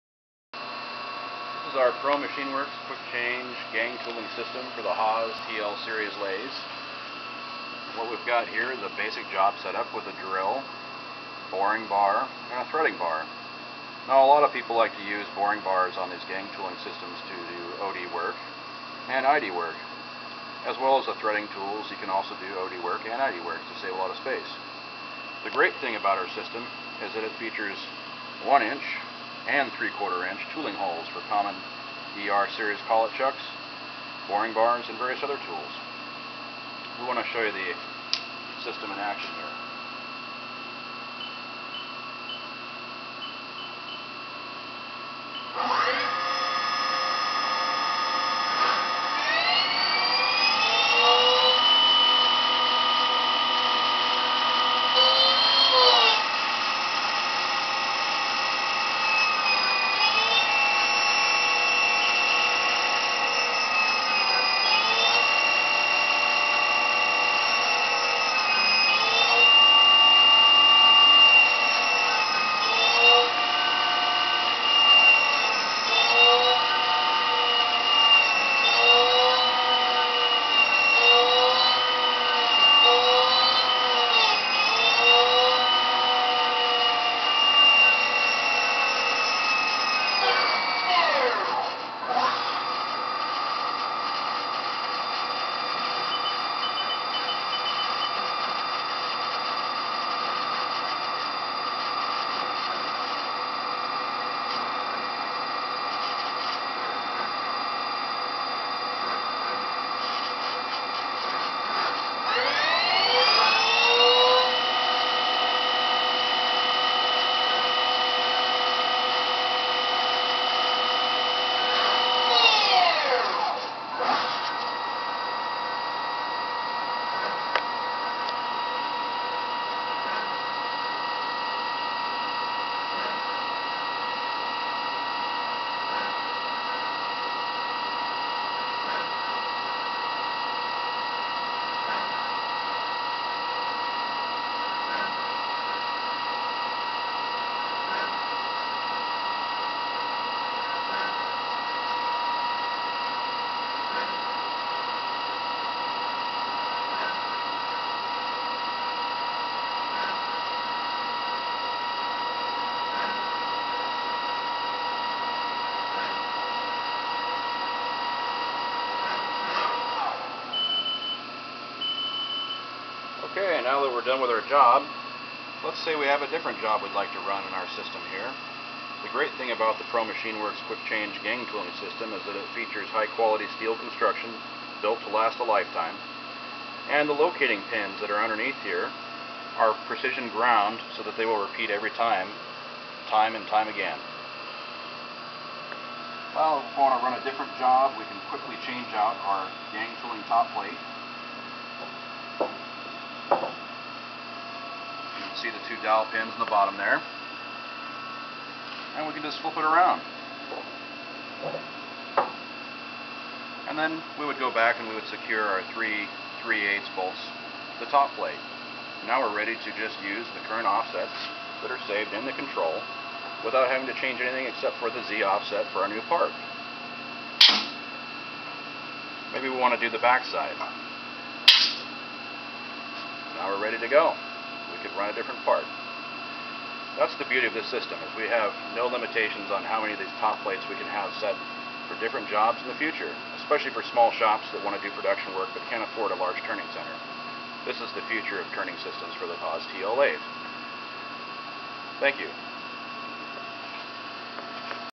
Quick Change Gang Tooling System for a Haas TL-1 and TL-2 CNC lathe
This is a quick video of the Pro Machine Works quick change gang tooling system for Haas TL-1 and TL-2 CNC lathes. All tooling systems are custom made to fit your Haas TL lathe. In order to properly fit the gang tool system to your lathe, we will need an accurate center height measurement from the top of the cross slide to the center of the spindle.